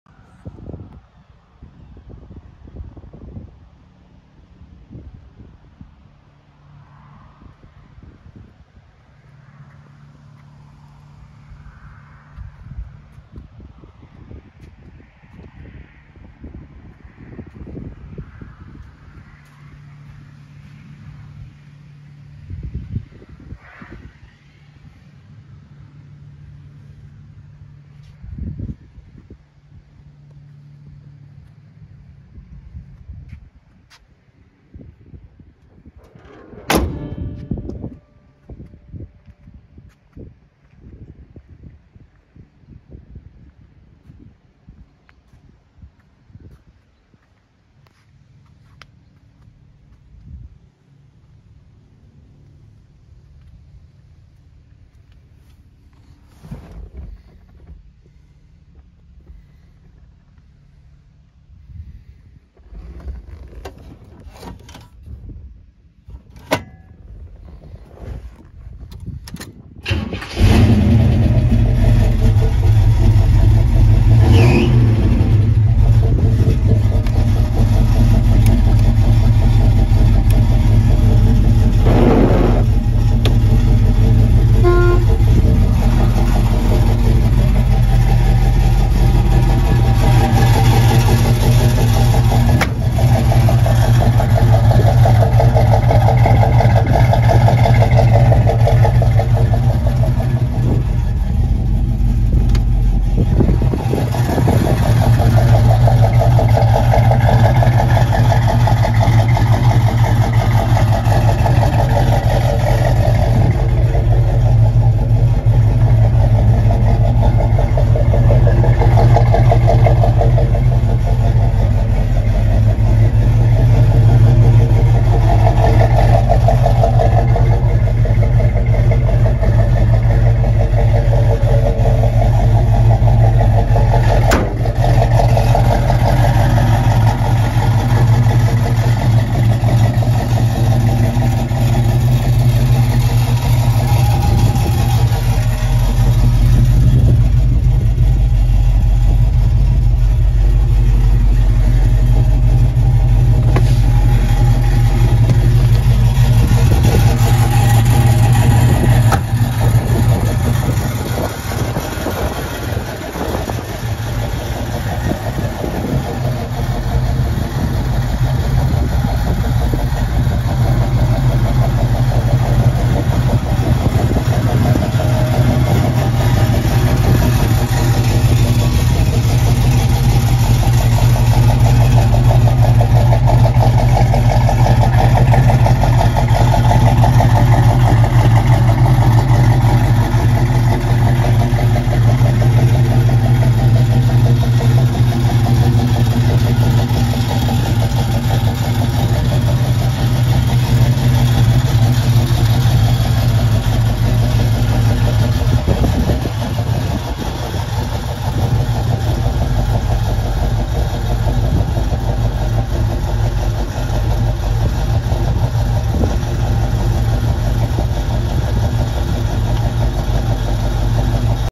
1969 CHEVROLET CAMARO SS L35 396/325HP
1969 Chevrolet Camaro SS, L35 396/325 hp, number matching, factory A/C, black deluxe interior, ps, pfdb, 3:73 12-bolt rear end. For more information and availability please visit our